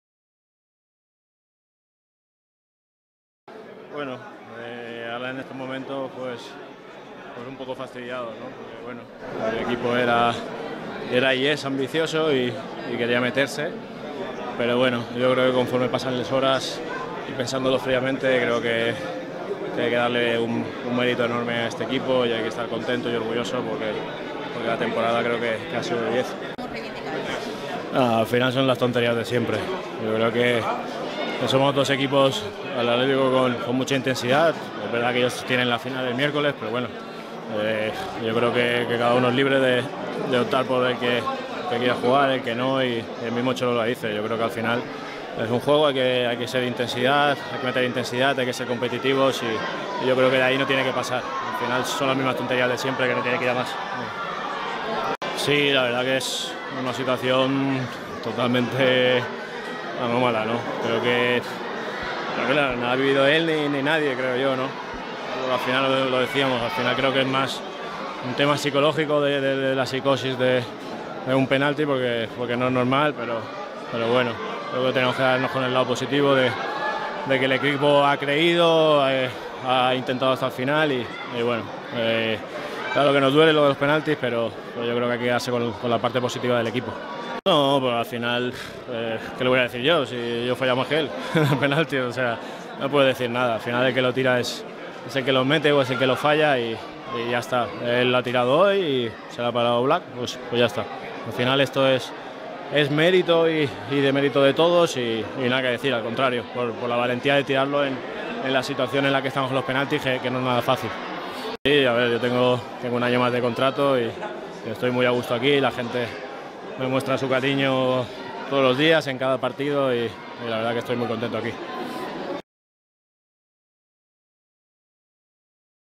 JORGE MOLINA 12 05 18
Jorge Molina, el capitán del Getafe, atendía a los medios de comunicación.